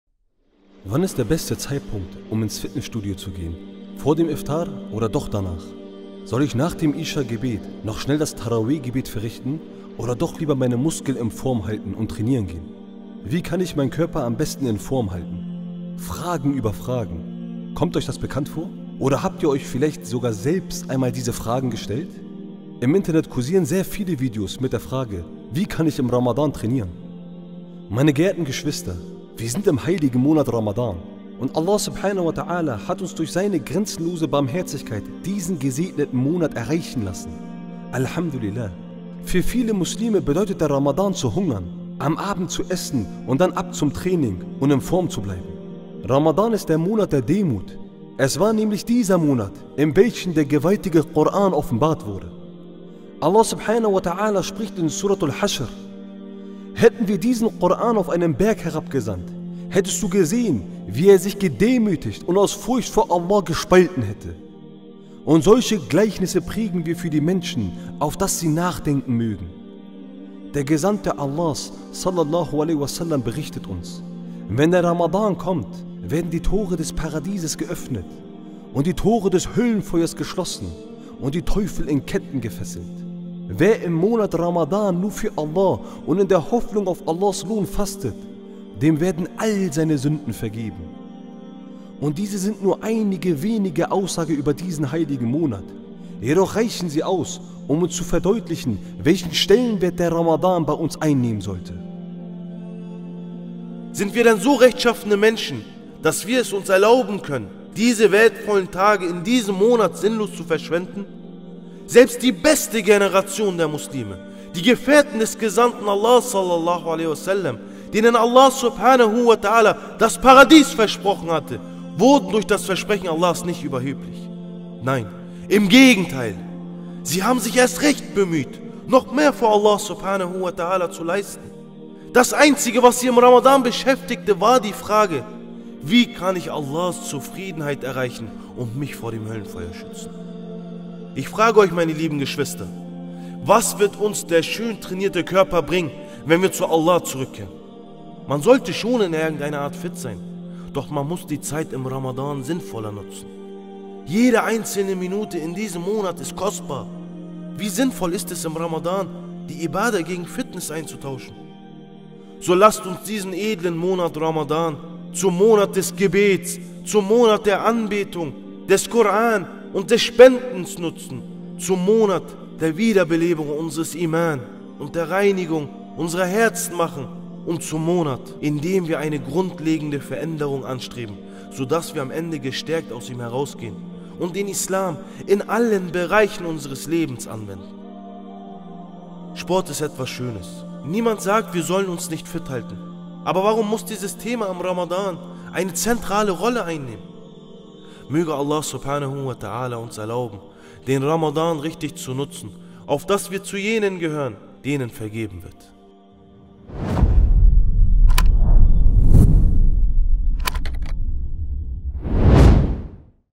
Wie mache ich Fitness an Ramadan? ᴴᴰ ┇ Worte zum Nachdenken ┇ BDI
„Wann ist der beste Zeitpunkt, um ins Fitnessstudio zu gehen? Vor dem Iftar oder doch danach?“ „Soll ich nach dem Isha Gebet noch schnell das Tarawih Gebet verrichten oder doch lieber meine Muskeln in Form halten und trainieren gehen?“